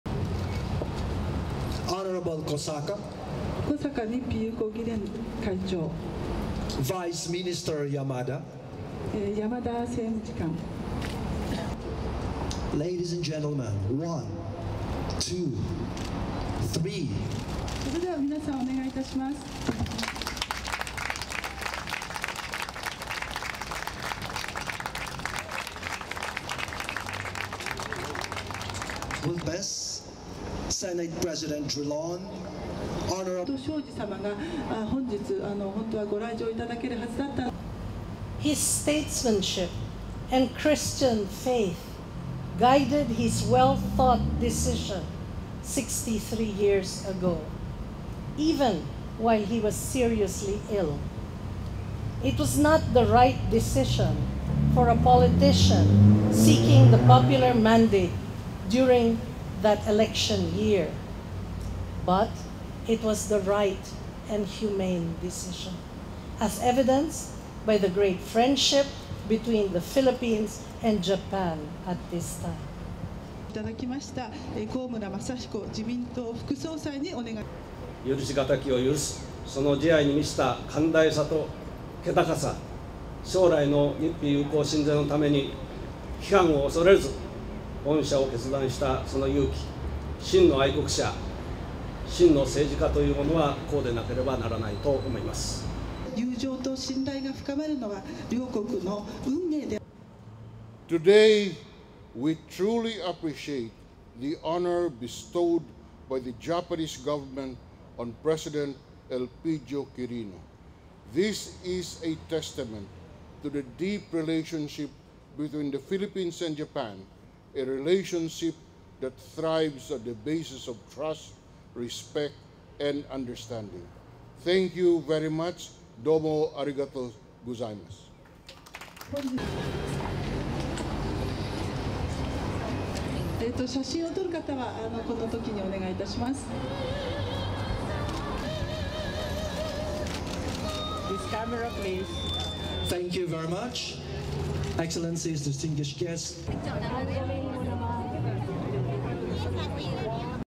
Memorial to late Philippines President Quirino
(18 Jun 2016) RESTRICTION SUMMARY: AP CLIENTS ONLY

Tokyo - 18 June 2016
1. Tilt up from Ruby Quirino Gonzalez-Meyer, granddaughter of former Philippine president Elpidio Quirino to Manuel Lopez, Ambassador of the Republic of the Philippines to Japan, then pan to Franklin Drilon, President of the Senate of the Philippines
2. Wide of unveiling ceremony
3. Close of Gonzalez-Meyer clapping
4. Close of portrait of former Philippine president Elpidio Quirino
5. Wide of Gonzalez-Meyer speaking
6. SOUNBITE (English) Ruby Quirino Gonzalez-Meyer, granddaughter of President Elpidio Quirino:
"His statesmanship and Christian faith guided his well-thought decision 63 years ago, even when he was seriously ill. It was not the right decision for a politician seeking the popular mandate during that election year, but it was the right and humane decision, as evidenced by the great friendship between the Philippines and Japan at this time."
7. Komura walking in for his speech
8. SOUNDBITE (Japanese) Masahiko Komura, Vice President of the Liberal Democratic Party:
“I must praise his merciful nobleness for his generosity to forgive the unforgivable, and for his courage to grant amnesty without fear of criticism for the sake of Japan-Philippine friendship in the future, as such should be the attributes of a true patriot and a true politician.”
9. Franklin Drilon, President of the Senate of the Philippines, approaching lectern
10. SOUNDBITE (English) Franklin Drilon, President of the Senate of the Philippines:
"Today, we truly appreciate the honour bestowed by the Japanese government on President Elpidio Quirino. This is a testament to the deep relationship between the Philippines and Japan - a relationship that thrives on the basis of trust, respect and understanding. Thank you very much."
11. Close of memorial
12. Various of photo opportunity
13. Close of engraved words on memorial
STORYLINE:
A memorial to former Philippines President Elpidio Quirino was unveiled in Tokyo on Saturday to recognise his contribution to Filipino-Japanese relations.
More than 200 people attended the ceremony in Tokyo including Quirino's granddaughter, Philippine Senate President Franklin Drilon and Masahiko Komura, Vice President of Japan's Liberal Democratic Party.
Quirino served as a president from 1948 to 1953 and oversaw Philippine reconstruction after World War II.
He pardoned 105 Japanese prisoner of wars in 1953 and paved a path toward the normalisation of Japan-Phillipines relations.